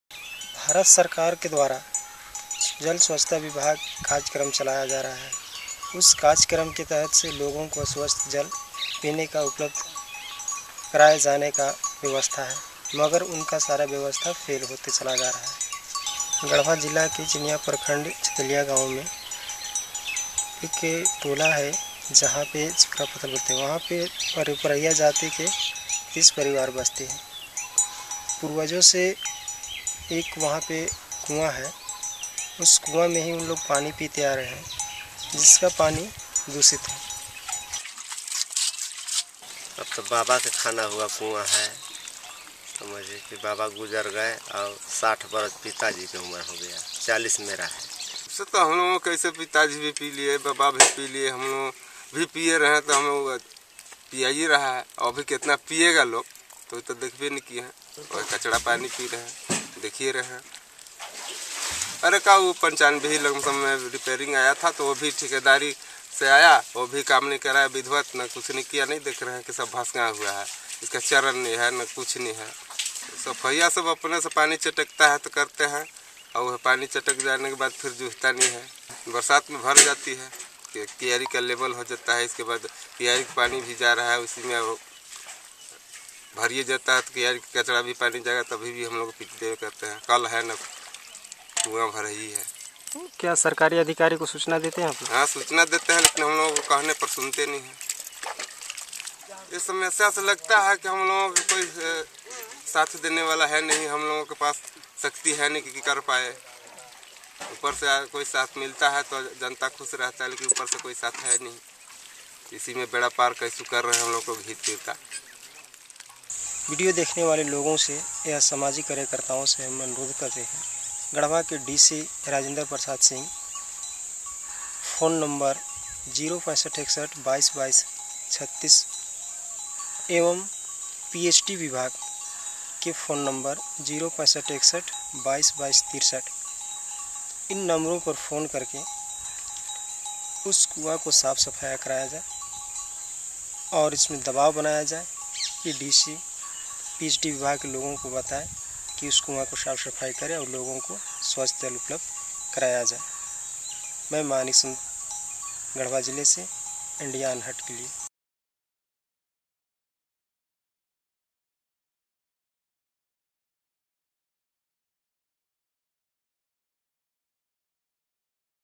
Bad Water Well in Chitalia, Garhwa, Jharkhand - Video Volunteer Manikchand Korwa Reports
This video was made by a Video Volunteers Community Correspondent Manikchand Korwa.